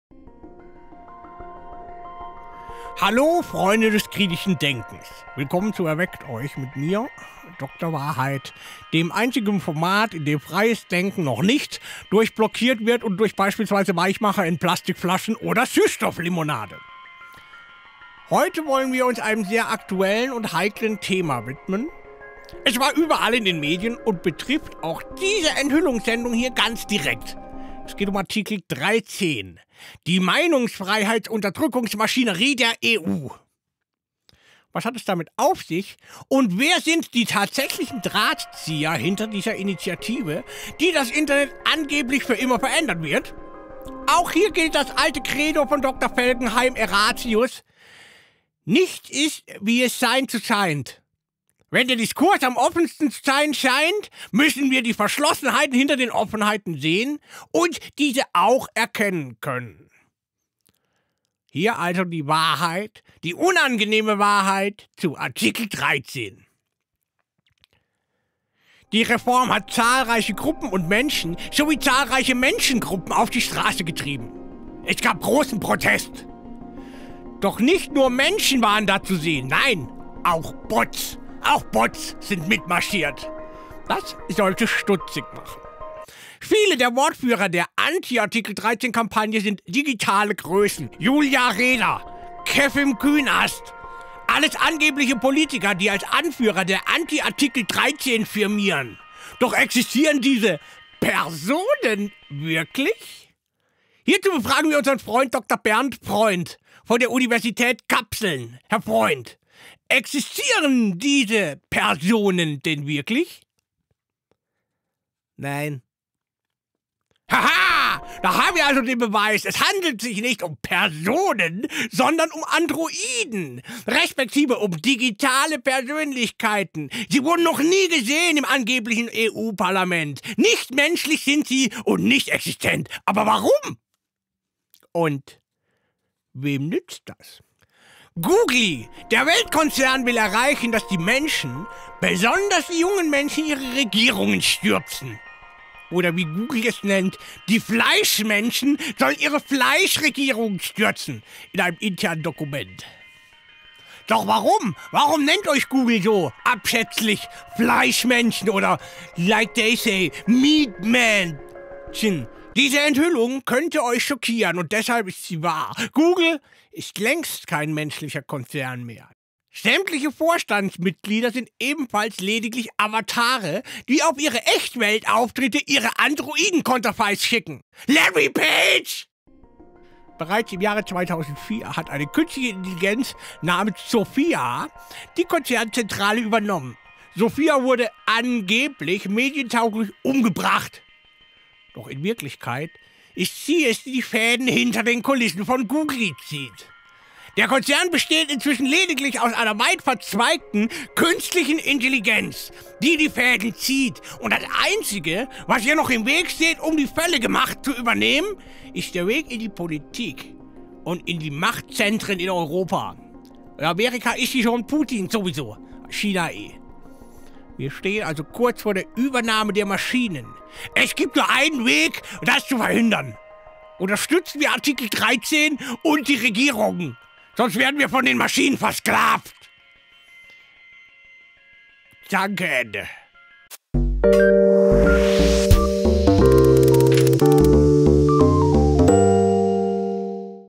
DIE WAHRHEIT ÜBER ARTIKEL 13 - ERWECKT EUCH! mit Dr. Wahrheit
Die schockierende(!!) Wahrheit über ARTIKEL 13 und die Drahtzieher hinter der EU-Reform. Was hat es mit dem angeblichen Urheberrecht wirklich auf sind und wer sind die Hintermänner und Drahtzieher? Dr. Wahrheit - die letzte Bastion des kritischen Denkens deckt überraschende Zusammenhänge auf!

ERWECKT EUCH! BLEIBT WACHSAM!

Dr. Wahrheit ist das letzte freie Medium im Systeminternet und wird in seiner investigativen Arbeit unterstützt von der WunderTütenFabrik